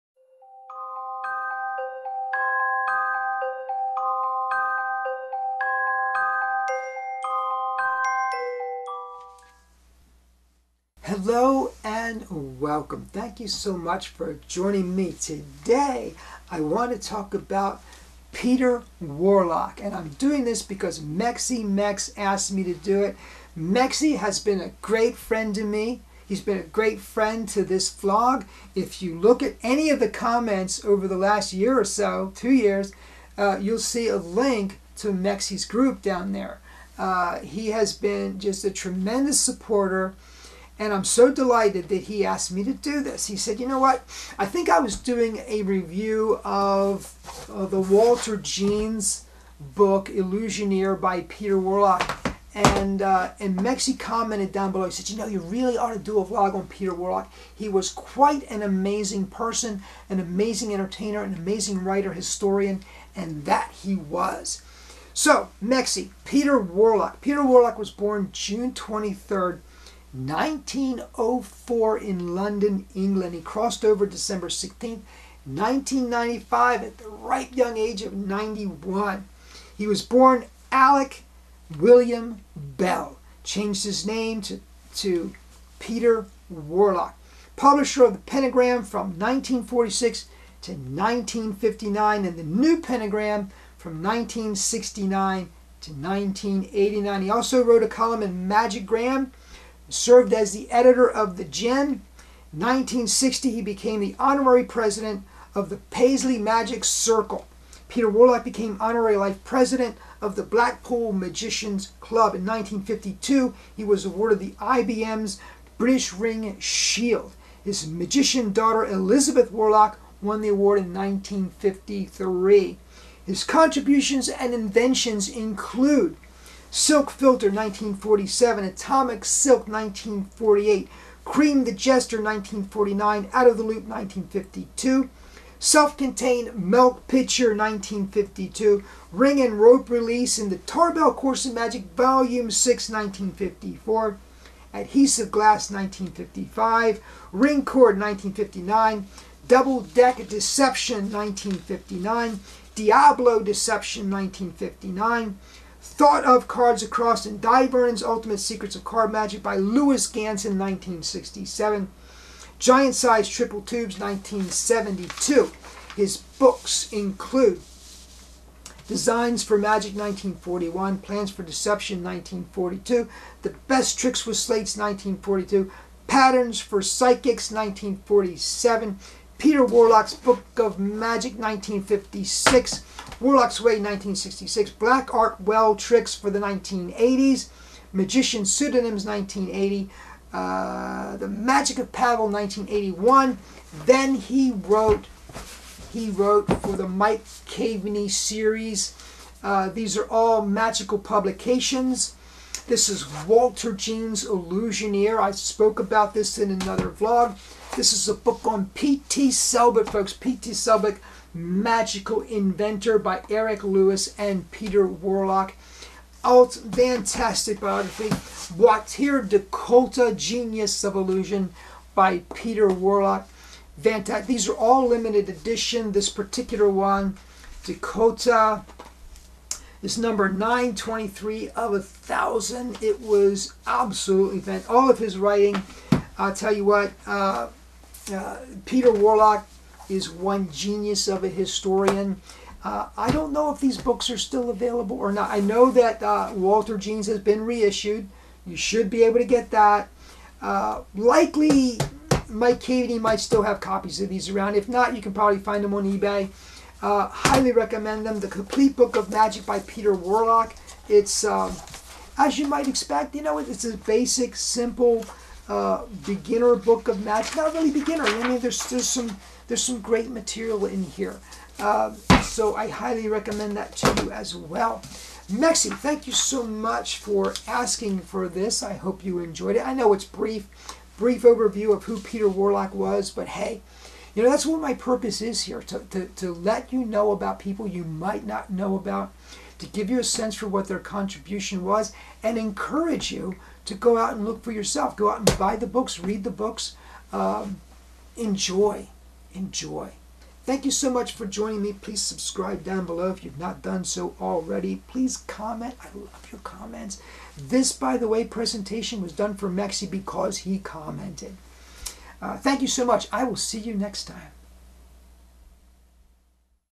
Peter Warlock
Today’s topic: Peter Warlock
For Mexie Mex
Peter Warlock June 23, 1904 London, England
Crossed over: December 16, 1995 (age 91)
Born: Alec William Bell
Publisher of The Pentagram from 1946 to 1959
The New Pentagram from 1969 to 1989.
Contributions and Inventions:
Silk Filter (1947)
Atomic Silk (1948)
Cream of the Jest (1949)
Out of the Loop (1952)
Self-Contained Milk Pitcher (1952)
Ring and Rope Release in Tarbell Course in Magic, Vol. 6 (1954)
Adhesive Glass (1955)
Ringcord (1959)
Double Deck Deception (1959)
Diabolo Deception (1959)
Thought of Card Across in Dai Vernon's Ultimate Secrets of Card Magic by Lewis Ganson (1967)
Giant-Size Triple Tubes (1972)
Books:
Designs for Magic (1941)
Plans for Deception (1942)
The Best Tricks with Slates (1942)
Patterns for Psychics (1947)
Peter Warlock's Book of Magic (1956)
Warlock's Way (1966)
Black Art Well Tricks for the 80s
Magicians' Pseudonyms (1980)
The Magic of Pavel (1981)
Walter Jeans: Illusioneer (1986)
P.T. Selbit: Magical Innovator (with Eric Lewis) (1989)
Buatier de Kolta: Genius of Illusion (1993)
I hope you enjoy what you see here.
I’d love to hear from you.
Much love,
David Dellman